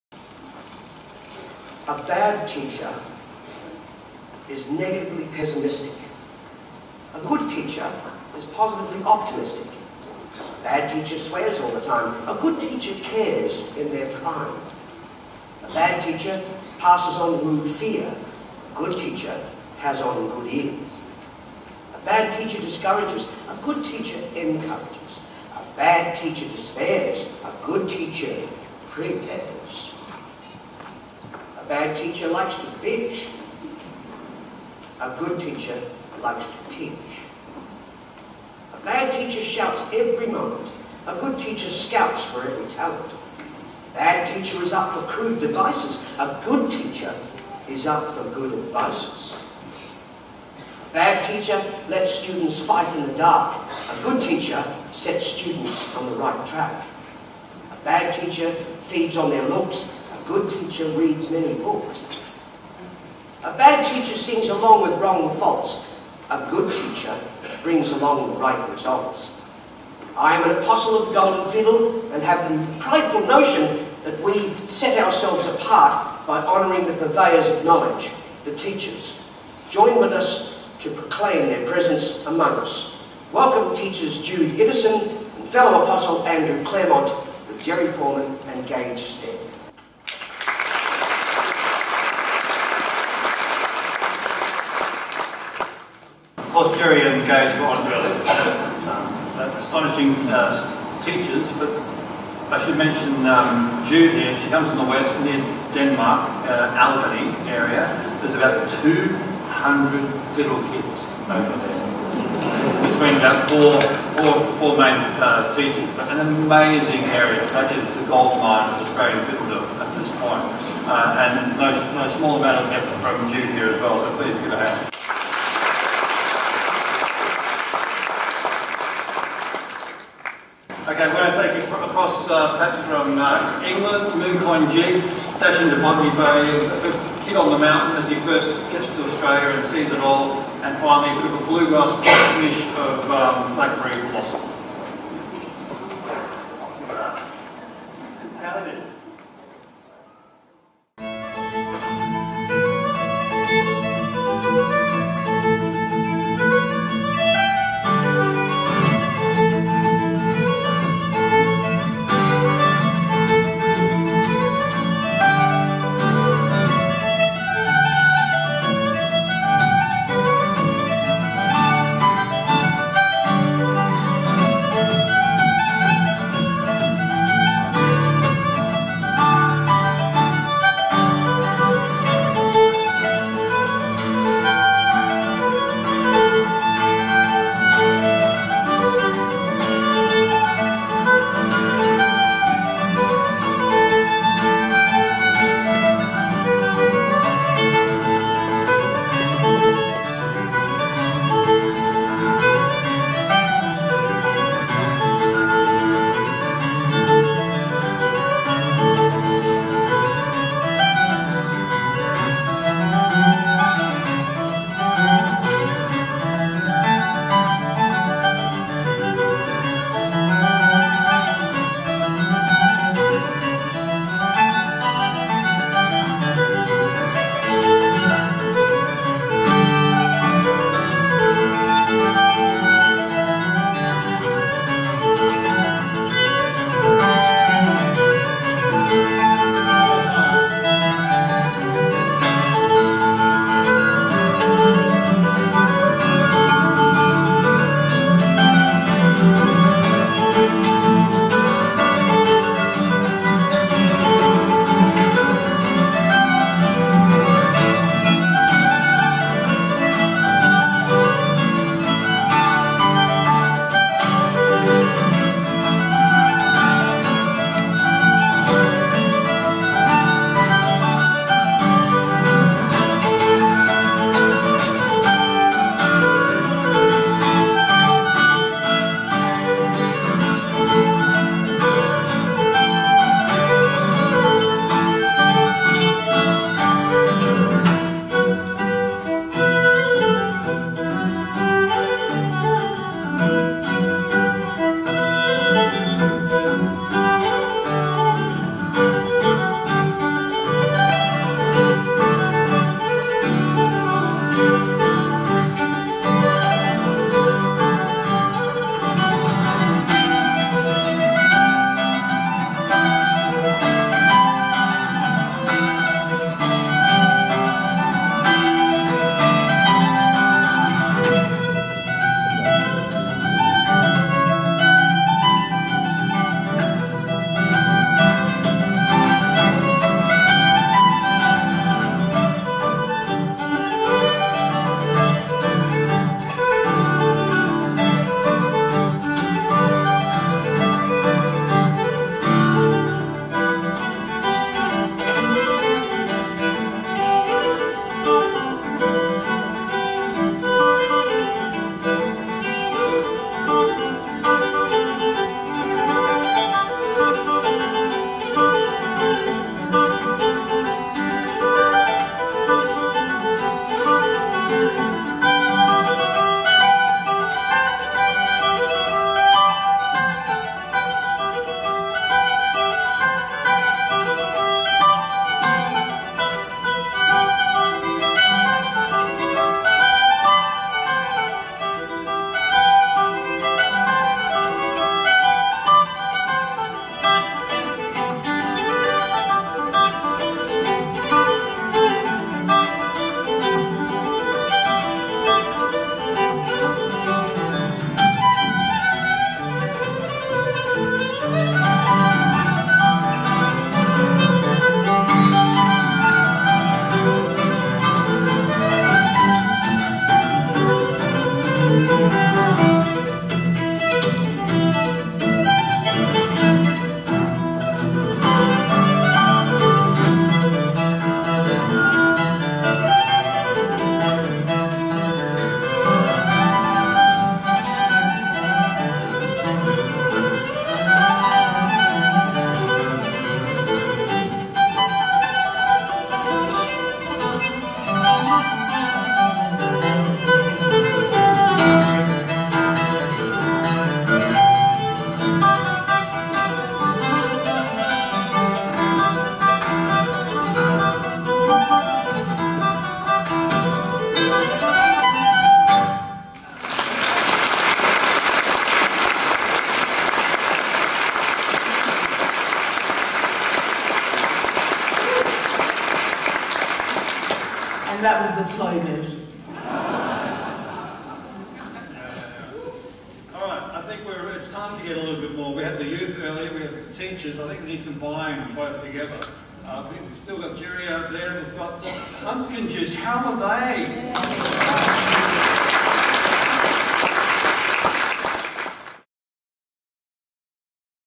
Jude Iddison & Andrew Clermont Golden Fiddle Show 2015
A fun frolic of tunes from UK through the Appalachian valleys to Botany Bay :-)